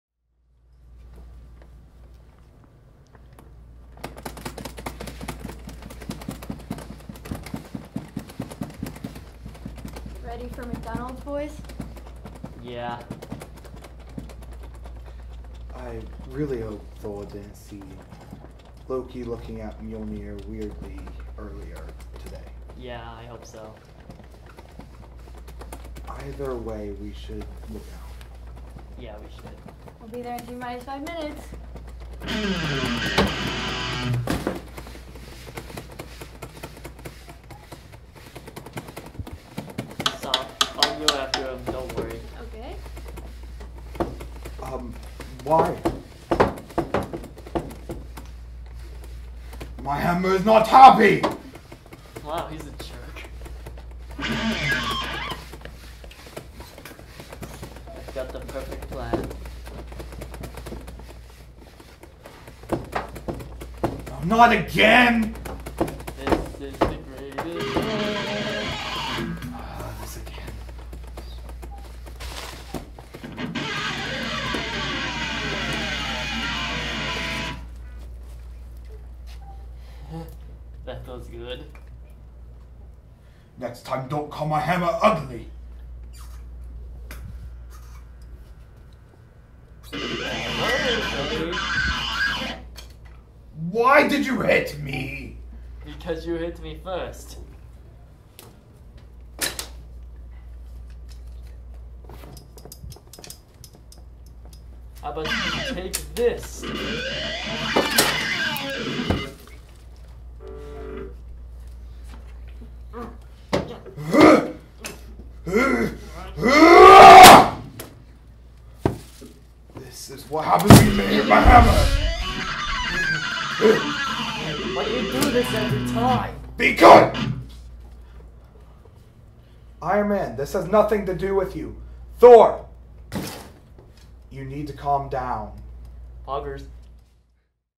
2023 HORNS Foley Challenge: Sky Smurfs (BTS)
The BeCre Foley challenge is a yearly activity in which the campers are split into two teams and given a random movie or video clip that has no sound. Then, using items found around the camp, the campers must become Foley artists and recreate the sounds from scratch without knowing what they sounded like in the first place. The challenge is fun, and the results can be incredibly funny.

This is the Behind-the-Scenes version of the challenge, with a live camera feed showing how the campers recreated the sound effects.

Video Clip from "The Avengers"
Sky Smurfs' video from 2023 HORNS at Butler Springs